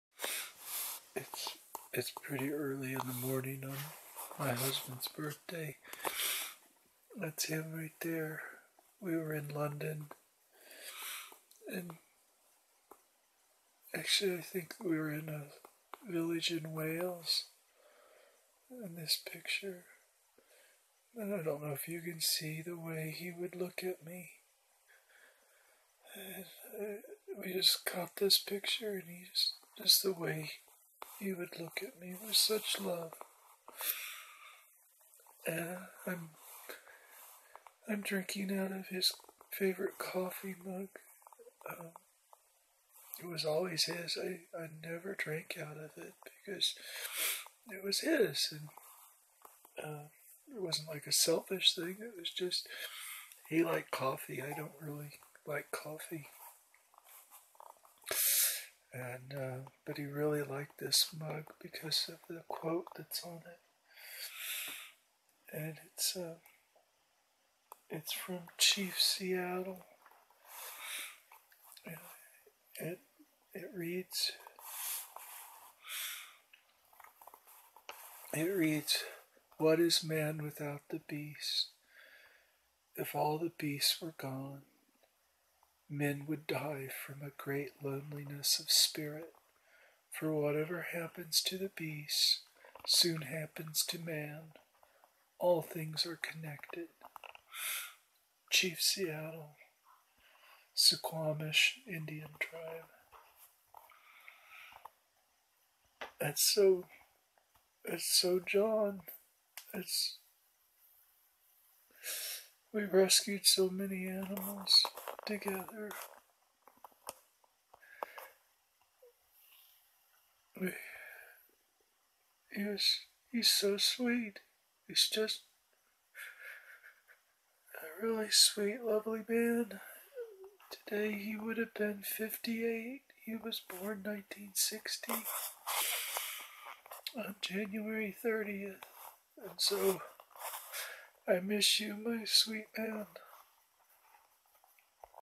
John's Birthday and favorite quote
Missing my sweet husband early in the morning on his birthday.  His first birthday since he died in my arms in this bed.  He is such a positive force and sweet spirit.